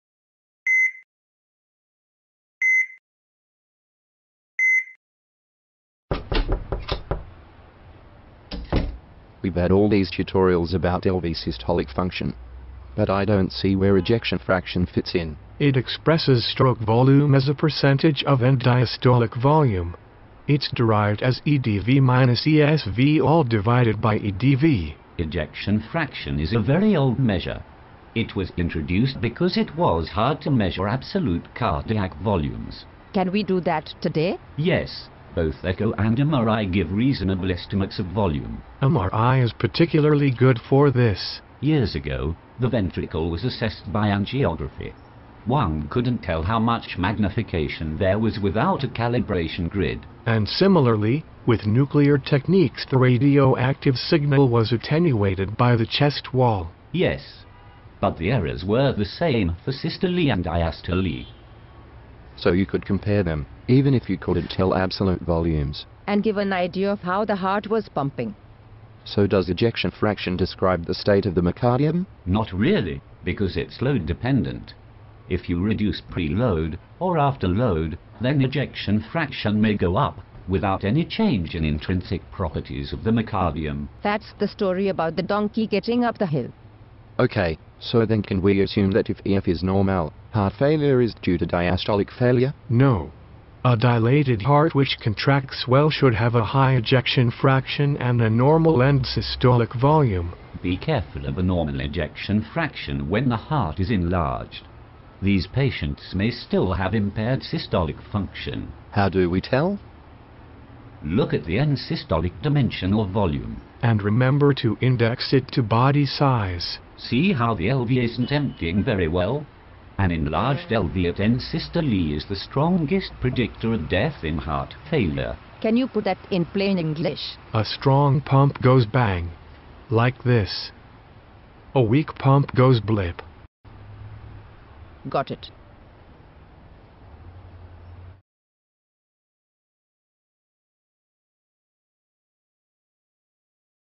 Ejection Fraction and Endsystolic Volume
More about the left ventricle in systole. EF is an old measure correcting for errors in absolute volume estimation.
From series 1 of Prof Montage Cardiology Rounds.